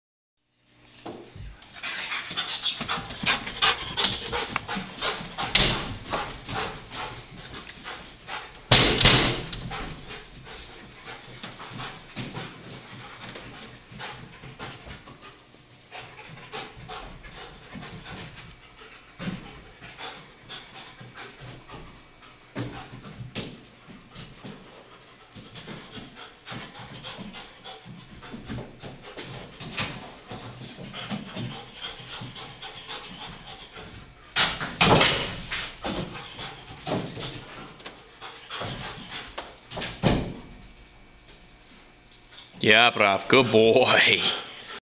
Enzo hunt inside 2
14 month old malinois learning to search inside